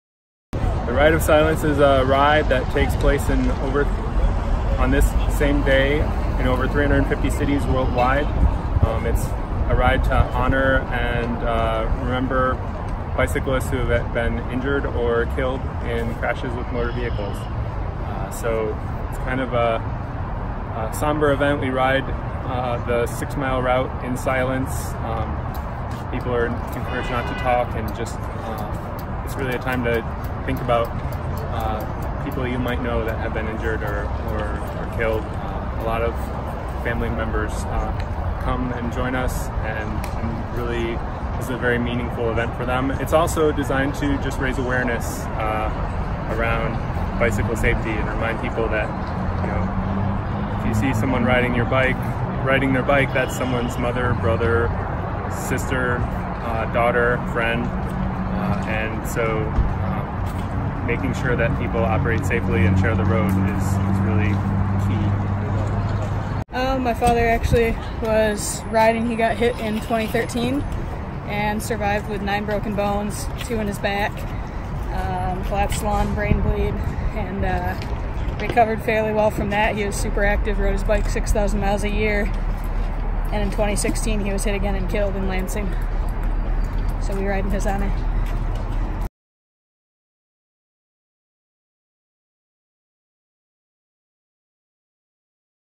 Grand Rapids bicyclists ride in silence to honor those killed on the road
The 2019 “Ride of Silence” brought more than 100 bicyclists to Grand Rapids to raise bike safety awareness and honor those killed on the road.